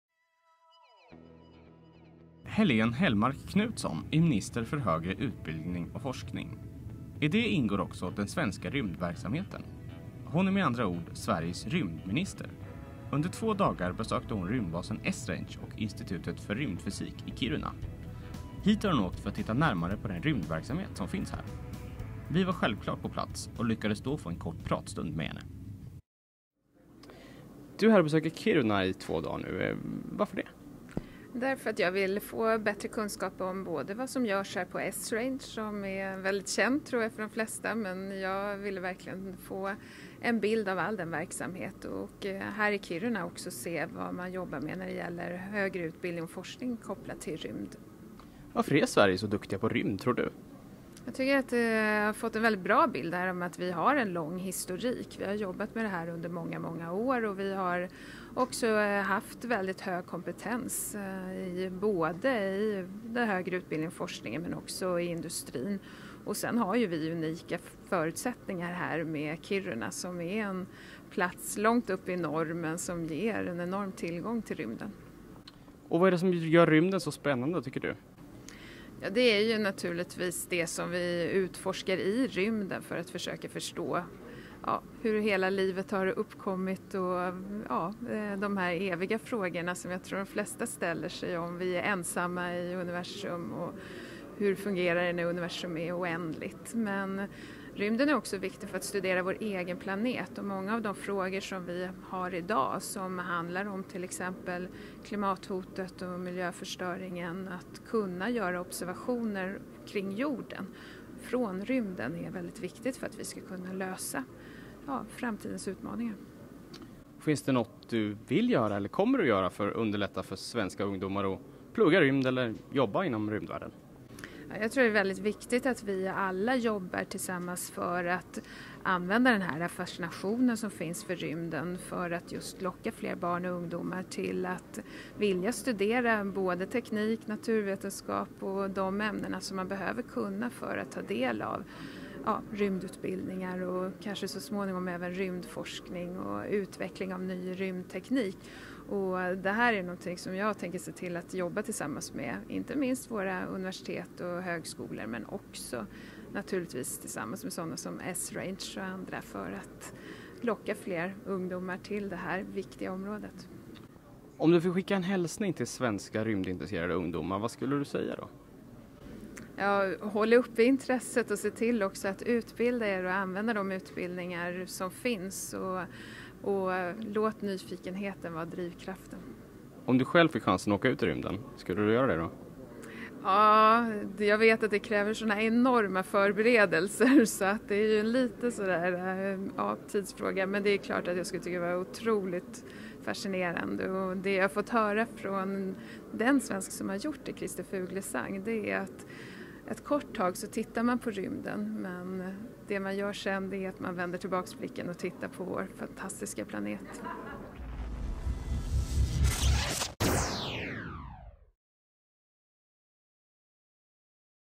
Rymdminister på besök i Kiruna
Helene Hellmark Knutsson är minister för högre utbildning och forskning. Det gör henne också till rymdminister! Vi träffade henne i Kiruna när  hon var där och besökte rymdbasen Esrange och Institutet för rymdfysik.